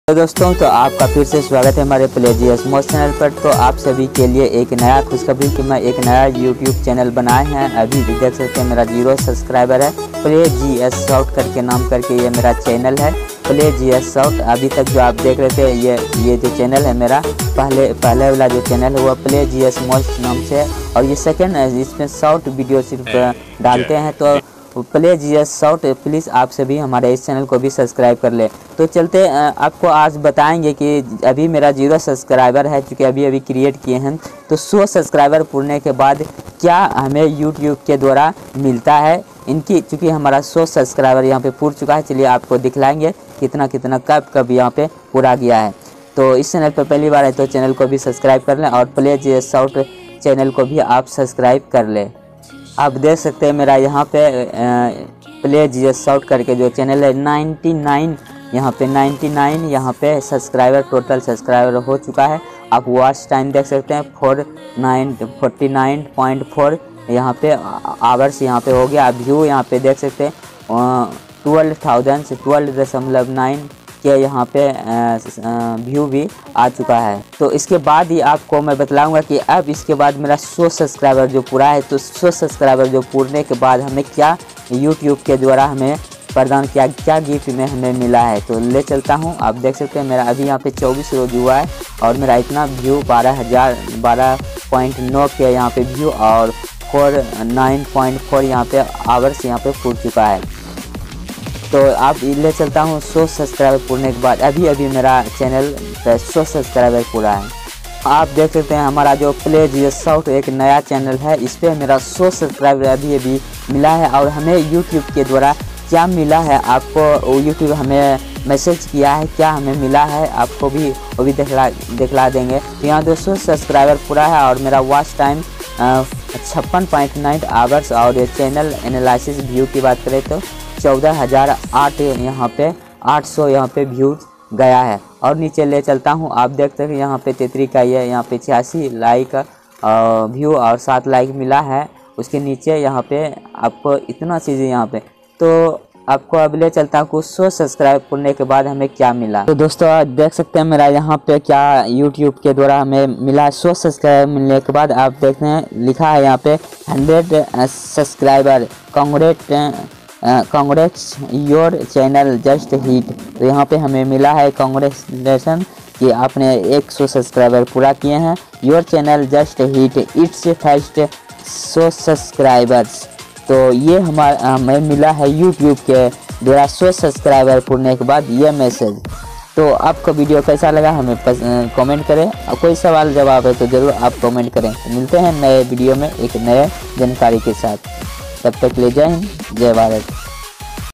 100 Subscribe पूरा करने पर हमें यूट्यूब के द्वारा मिला है..क्या आपको मिला है?playgsmost
100 Subscriber पूरा करने पर हमें यूट्यूब के द्वारा मिला है..क्या आपको मिला है?playgsmost
hello guess..
आज इस वीडियो में आप सभी को बताने जा रहा हूं कि 100 subscriber पूरा जैसे ही आपके चैनल पर होता है तो यूट्यूब के द्वारा आप को क्या देता है जरूर दोस्तों जैसे ही 100 subscribe आपके चैनल पर पूरा होगा तो यूट्यूब चैनल के द्वारा आप को एक गिफ्ट दिया जाता है क्या आपको गिफ्ट मिला है ! अगर नहीं मिला है तो इस वीडियो में जरूर देखें क्या कि वह गिफ्ट दिया जाता है यूट्यूब के द्वारा आपको 100s ubscriber पूरा करने पर तो पूरा वीडियो देखें और शुरू से अंत तक देखें skeep बिल्कुल ना करें तो hello  guess..
                    तो हमें एक ज सुंदर सा गिफ्ट मिला है जो की बहुत ही सुंदर है

1  to 7 days 100